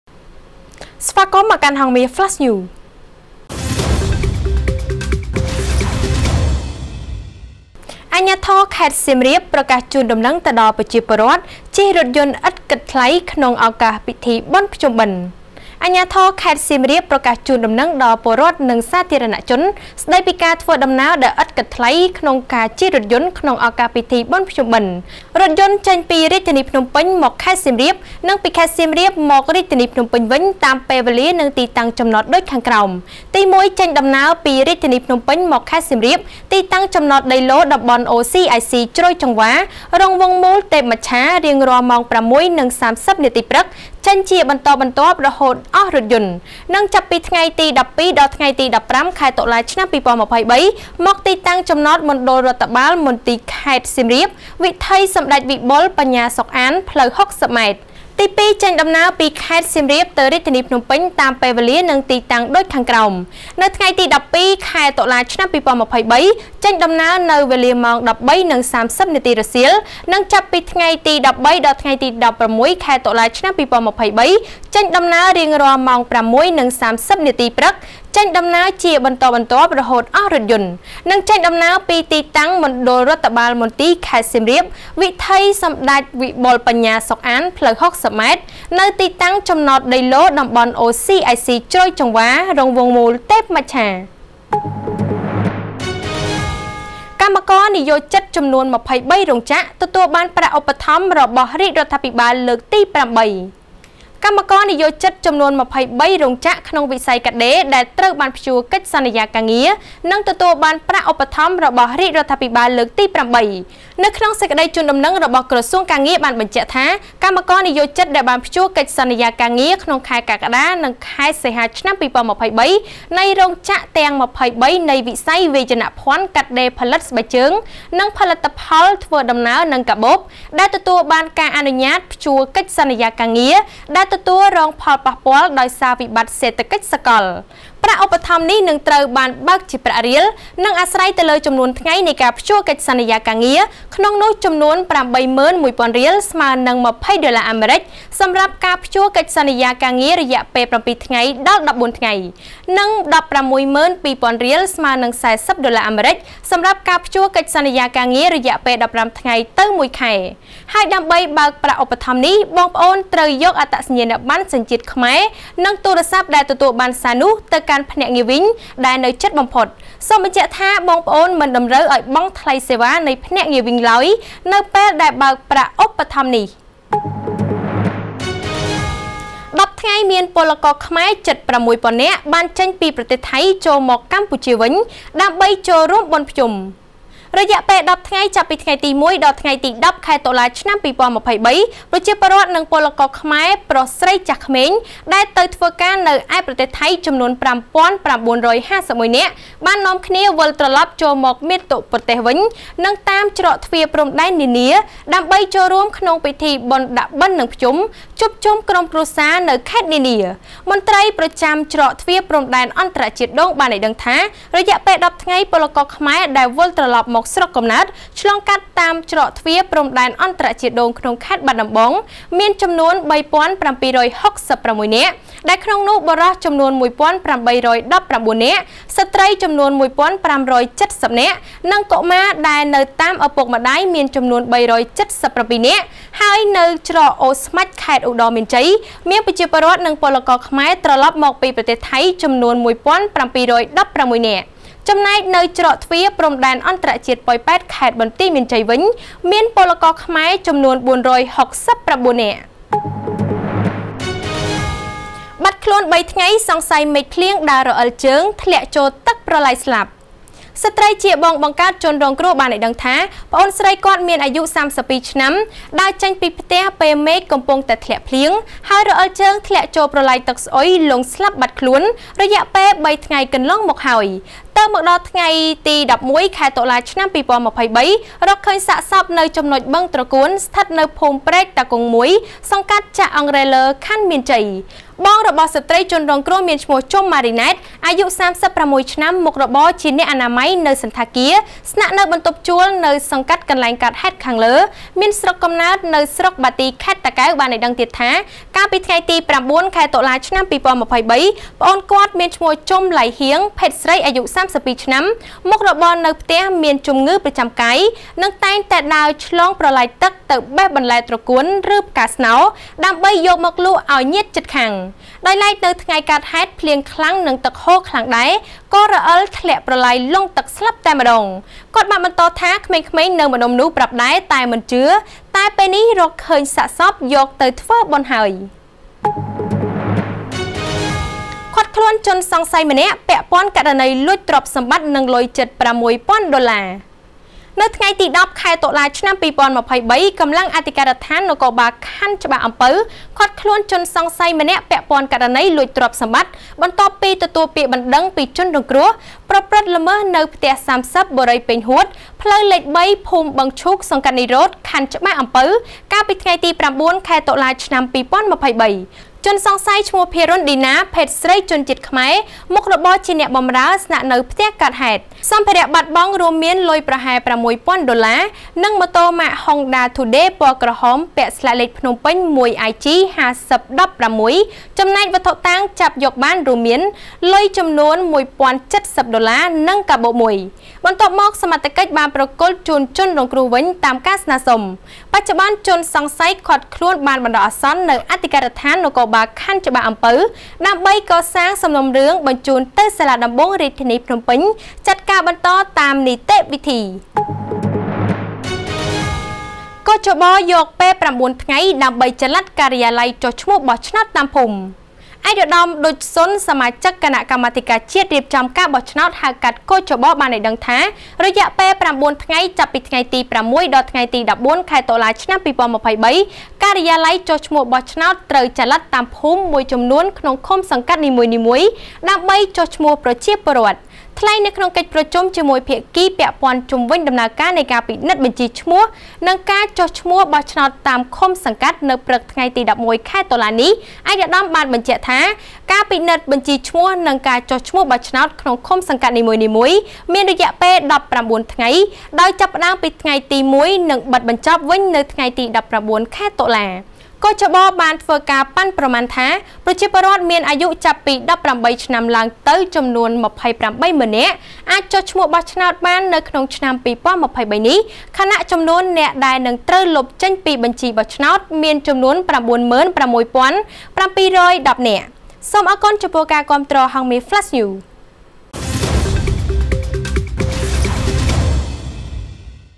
អាជ្ញាធរខេត្តសៀមរាប ប្រកាសជូនដំណឹងដល់ពលរដ្ឋ ជិះ...| Flash News (11-10-2023) 5:30PM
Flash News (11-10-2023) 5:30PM
▪ អាជ្ញាធរខេត្តសៀមរាប ប្រកាសជូនដំណឹងដល់ពលរដ្ឋ ជិះរថយន្ដឥតបង់ថ្លៃក្នុងឱកាសពិធីបុណ្យភ្ជុំបិណ្ឌ
▪ កម្មករ និយោជិតចំនួន២៣រោងចក្រ ទទួលបានប្រាក់ឧបត្ថម្ភ របស់រាជរដ្ឋាភិបាល លើកទី៨
▪ ១០ថ្ងៃ មានពលករខ្មែរជិត៦ពាន់នាក់ បានចេញពីប្រទេសថៃ ចូលមកកម្ពុជាវិញ ដើម្បីចូលរួមបុណ្យភ្ជុំ
▪ បាត់ខ្លួន៣ថ្ងៃ សង្ស័យមេឃភ្លៀងដើររអិលជើង ធ្លាក់ចូលទឹកប្រឡាយស្លា,ប់
▪ ឃាត់ខ្លួនជនសង្ស័យម្នាក់ ពាក់ព័ន្ធករណីលួចទ្រព្យសម្បត្តិនិងលុយជិត៦ពាន់ដុល្លារ
▪ គ.ជ.ប យកពេល៩ថ្ងៃ ដើម្បីចល័តការិយាល័យចុះឈ្មោះបោះឆ្នោត តាមភូមិ